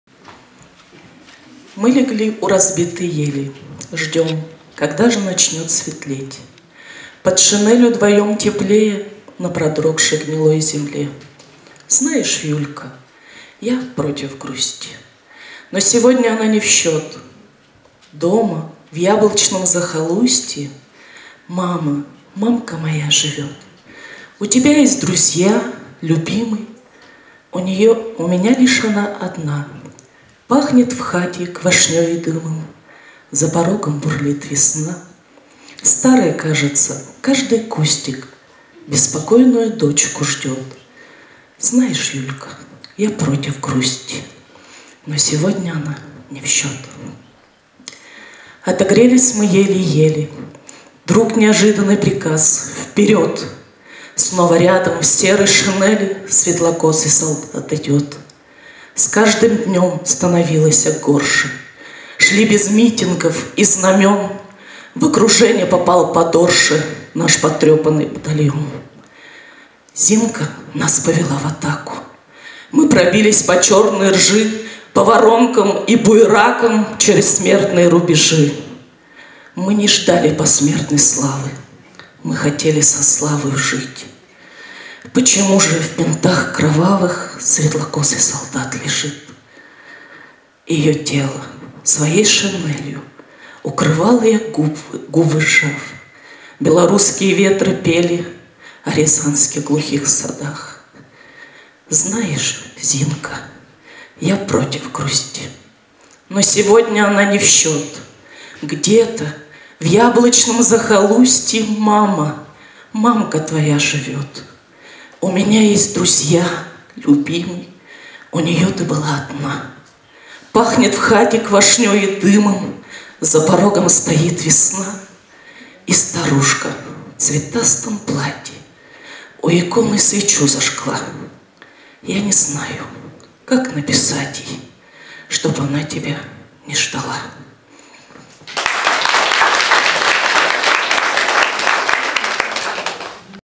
Стихотворение Зинка о войне
Стихотворение «Зинка», автор Юлия Друнина, читает Арестова Наталья, первичная профсоюзная организация в/ч 83243